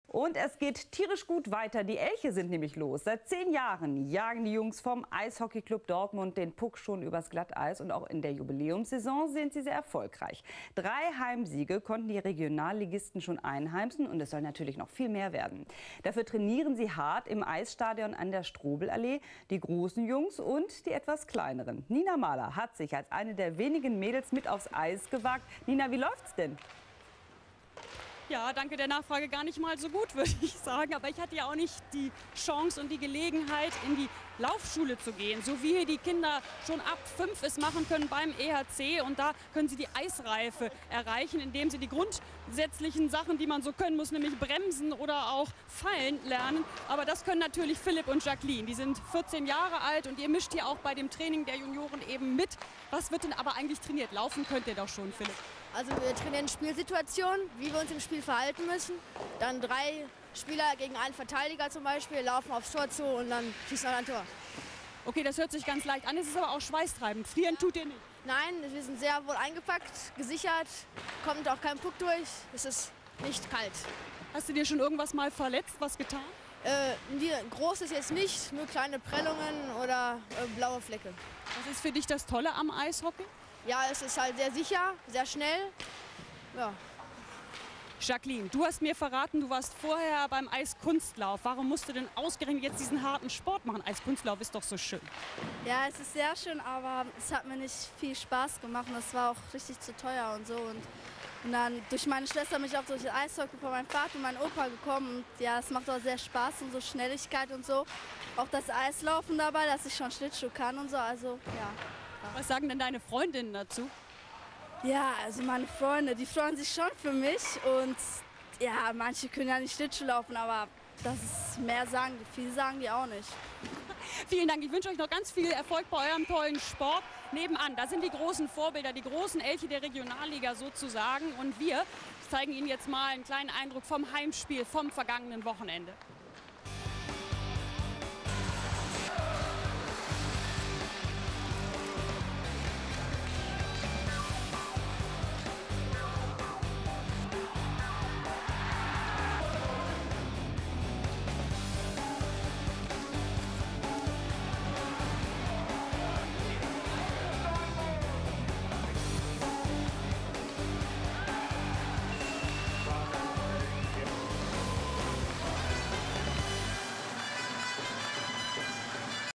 EHC Dortmund im WDR (1)
Eishockey in Dortmund - Fernsehbericht über die Elche